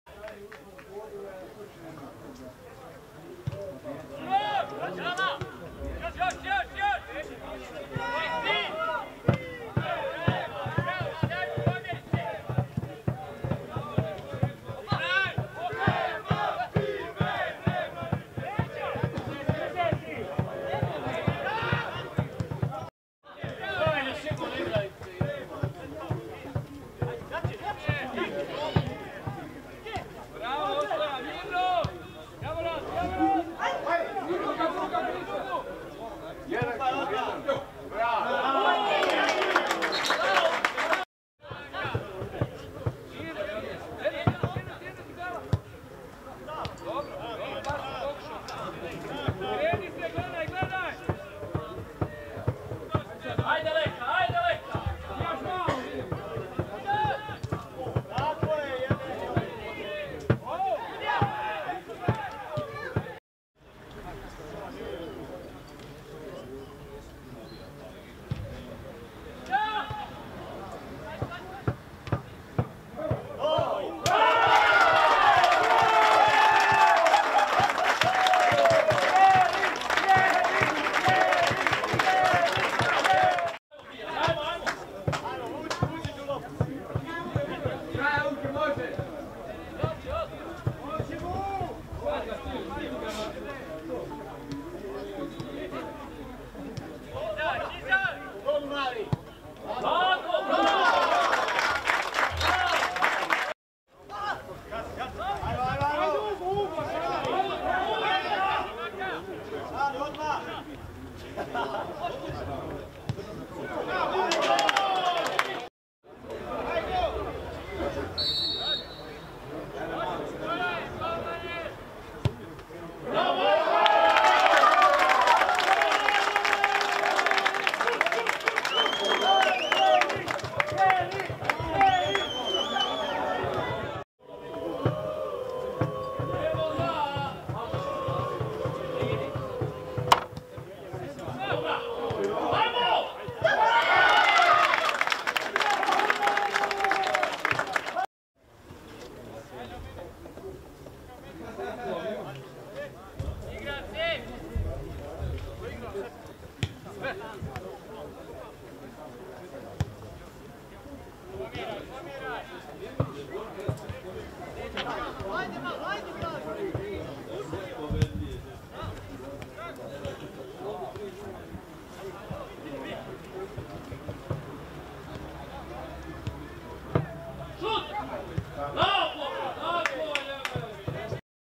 Laktaši - Sloga (T) 3:0 [HIGHLIGHTS] 17.8.2019.
Druga liga Republike Srpske / grupa Zapad / II kolo